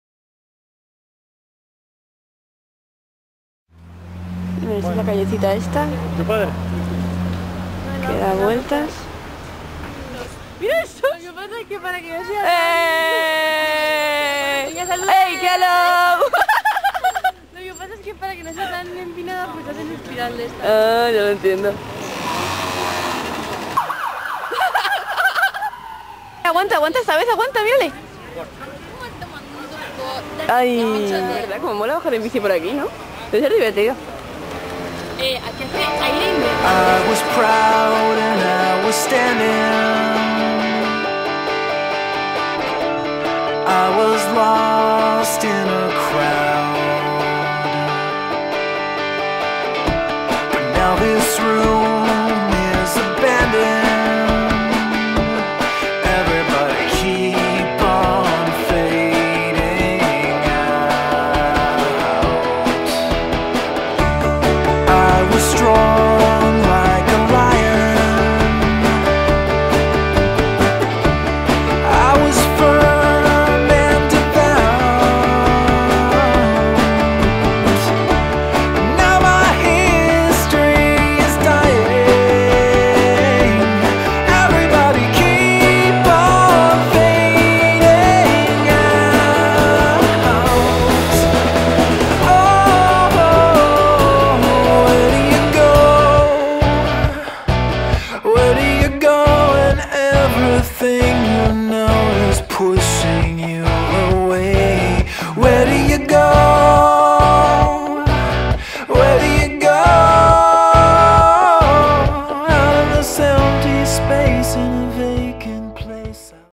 Calle Lombard en San Francisco - Driving by Lombard Street in San Francisco
Vídeo de las famosas curvas de la calle Lombard en San Francisco, California, con imágenes de la conducción en coche por bajando por sus empinadas curvas